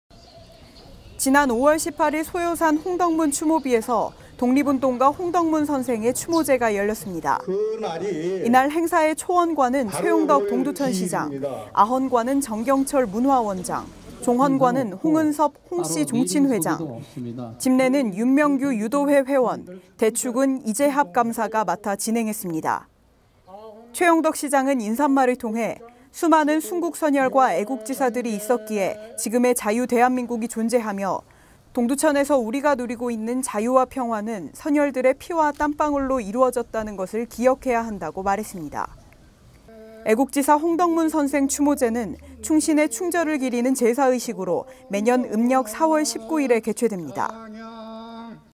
제7회 애국지사 홍덕문 선생 추모제
지난 5월 18일 소요산 홍덕문 추모비에서 독립운동가 홍덕문 선생의 추모제가 열렸습니다. 이날 행사의 초헌관은 최용덕 동두천시장, 아헌관은 정경철 문화원장, 종헌관은 홍은섭 홍씨종친회장, 집례는 윤명구 유도회회원, 대축은 이재합 감사가 맡아 진행했습니다.
최용덕 시장은 인사말을 통해 ‘수많은 순국선열과 애국지사들이 있었기에 지금의 자유대한민국이 존재하며, 동두천에서 우리가 누리고 있는 자유와 평화는 선열들의 피와 땀방울로 이루어졌다는 것을 기억해야 한다.’고 말했습니다.
애국지사 홍덕문 선생 추모제는 충신의 충절을 기리는 제사의식으로 매년 음력 4월 19일에 개최됩니다.